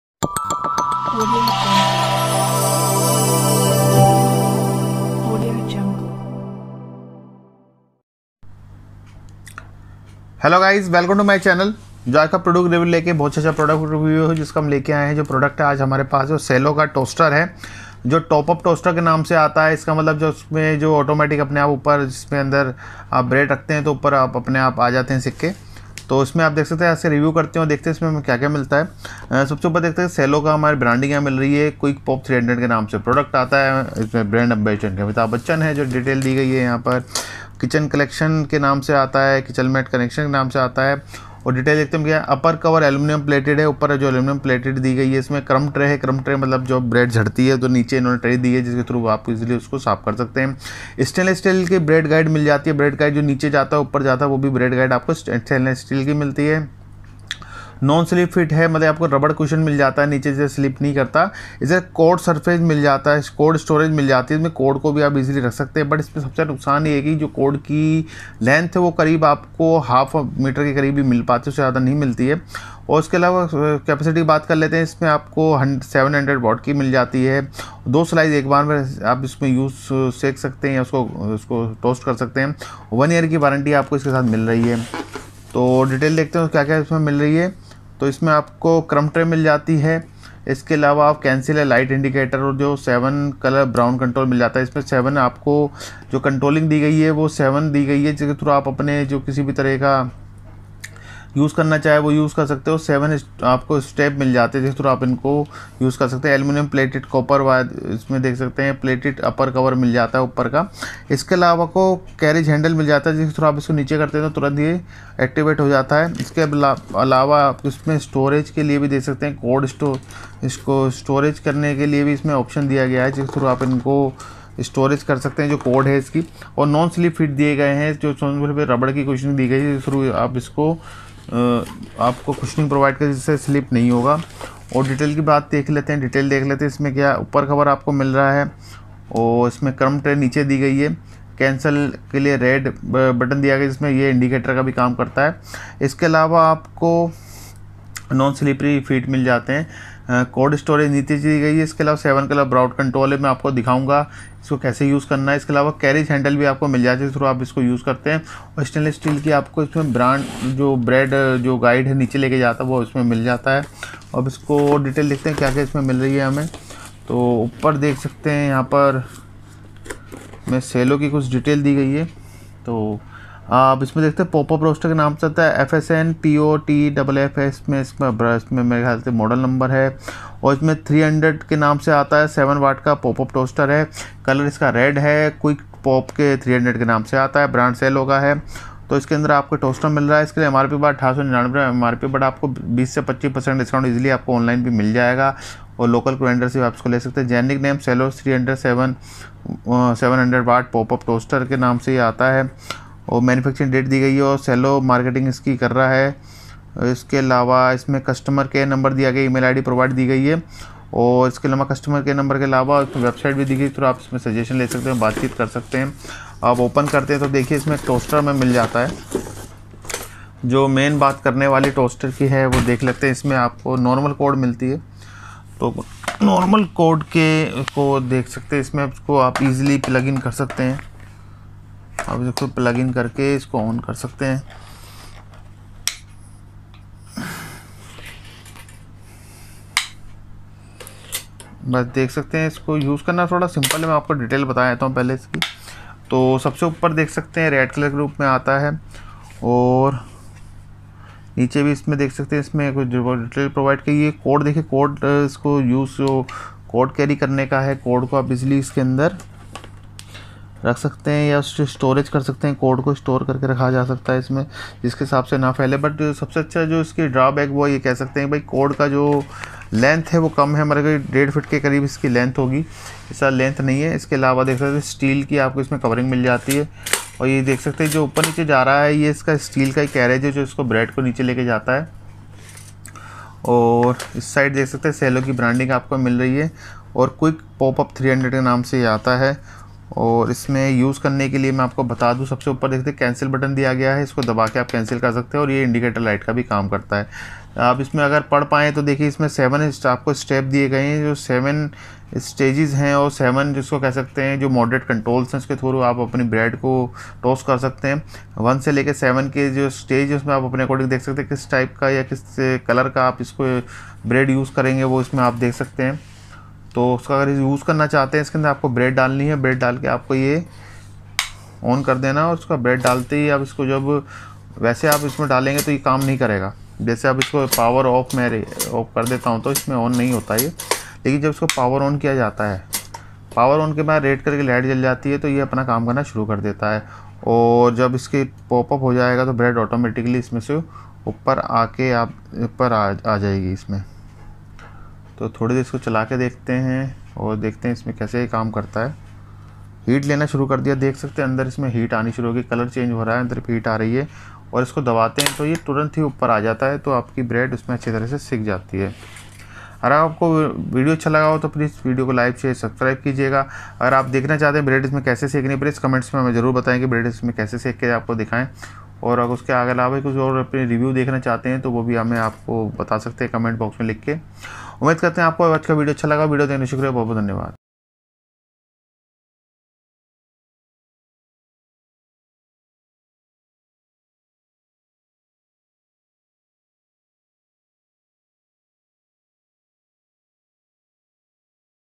Cello Quick 2 Slice Pop Up 300 Toaster (Red)
Product details

Brand

Cello

Colour

Red

Material

Plastic

Item Dimensions LxWxH

30 x 15.6 x 19 Centimeters

Item Weight

810 Grams

Wattage

700 Watts

Includes: Pop-up toaster,User Manual and Warranty card;

Carriage Handle and Cord storage facility

Crumb Tray

Stainless steel bread tray

Cello Quick 2 Slice Pop-up Toaster

Make crispy delicious toast with Cello's 2 slice toaster. The sleek small unit provides reliable results, toasting one or two slices of bread at a time. A useful addition to any kitchen counter, the two-slice toaster features a stunning red appearance for attractive everyday style.

7 Colour Brown Control

An adjustable knob lets you control the level of browness of the toasted bread. Choose lower settings for lighter toast or higher settings for darker, crispier toast.

Crumb Tray

A convenient pop out crumb tray at the bottom of the toaster collects fallen crumbs for easy cleaning.

'Cancel' and Red Light Indicator

A convenient 'cancel' button allows you to cancel the ongoing operation while a red light indicator alerts you when your toast is ready.

Cord Storage

The bottom of the toaster features a cord storage which lets you neatly wind up the toaster's cord when the unit is not in use.

Aluminium-Plated Upper Cover

The upper cover of the toaster is plated with aluminium for extra durability and easy maintenance.